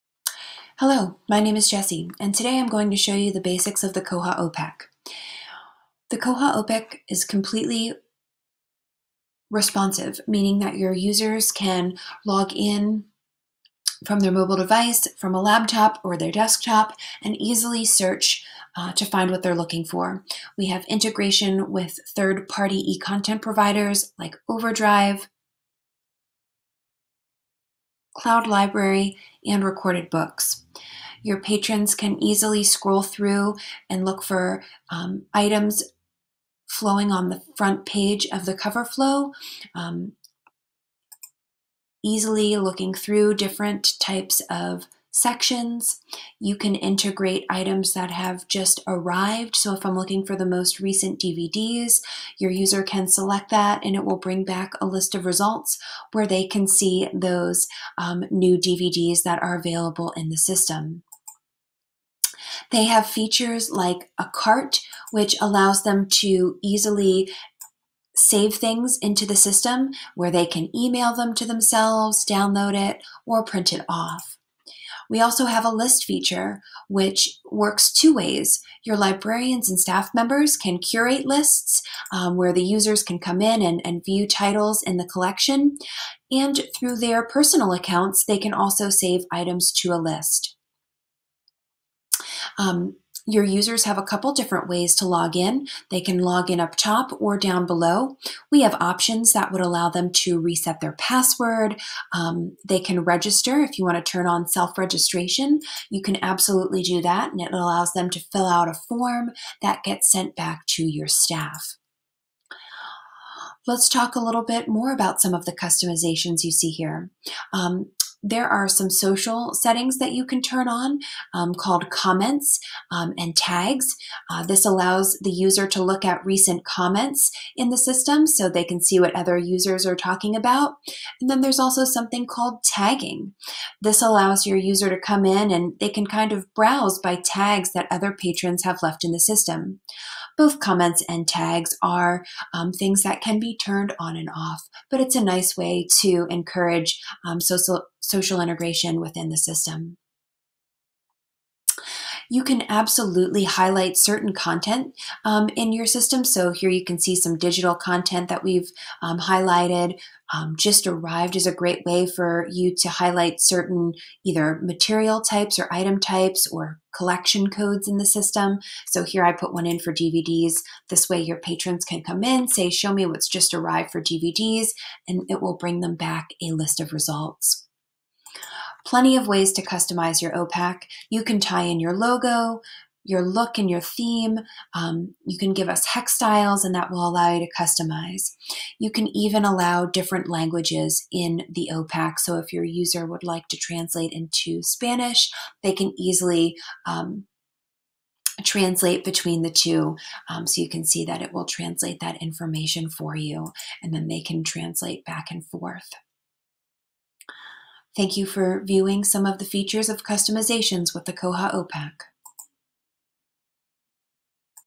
Koha - Customizable OPAC
Take a tour of the Koha OPAC and customizations you can make.